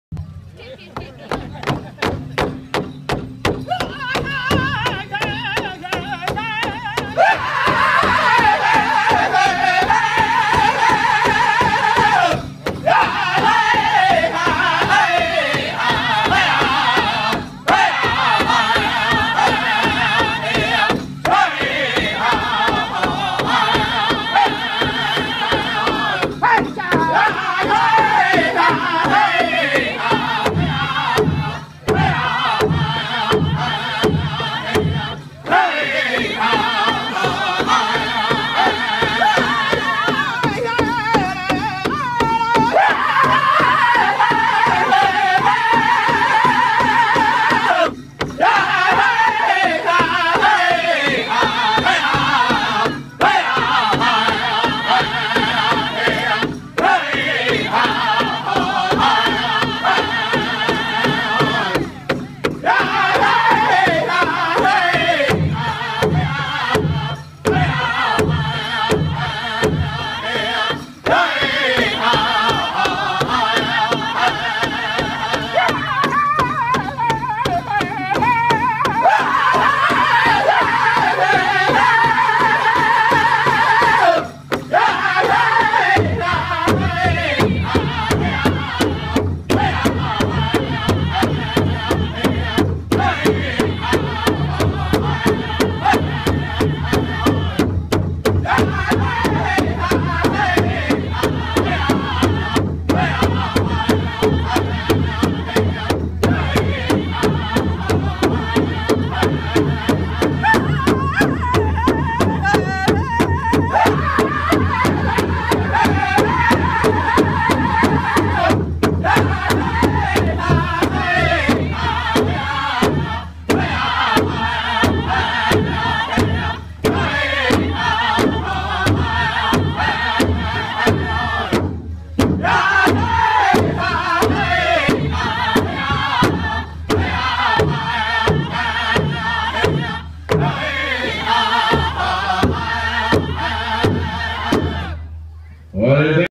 Young Kingbird Red Lake July 2015
Young Kingbird at the Red Lake Independence Day Celebration. July 2015. Friday night.